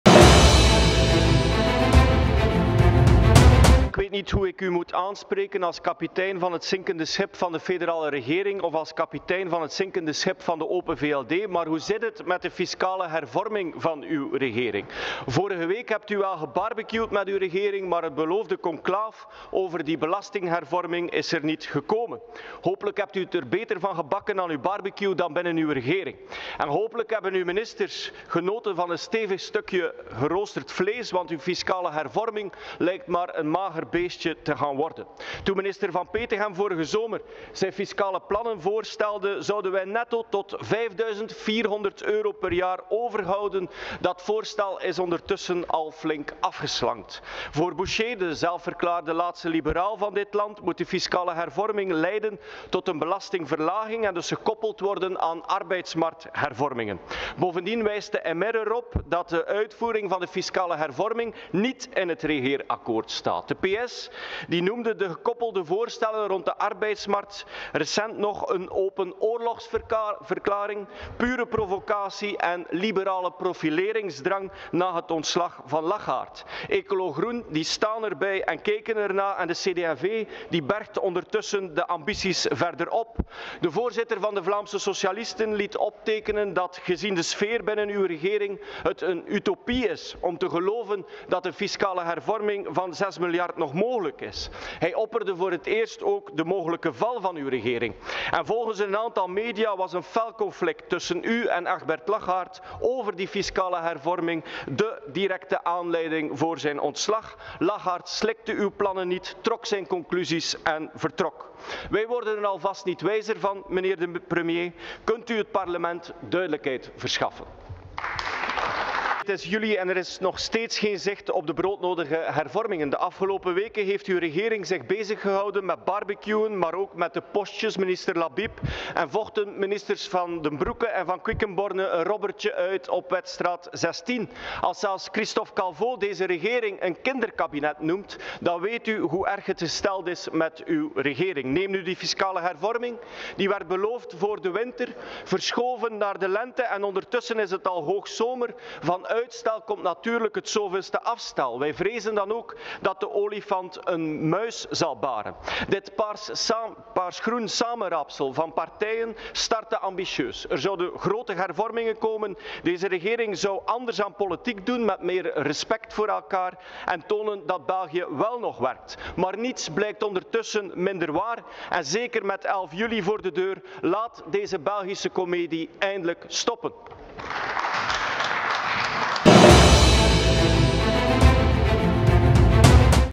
Waar blijft die fiscale hervorming?
Kamerlid Wouter Vermeersch op 6 juli 2023.

De fiscale hervorming werd beloofd voor de winter, uitgesteld naar de lente en nu we in de zomer zijn, is er nog steeds niets. Laten we deze Belgische komedie eindelijk stoppen!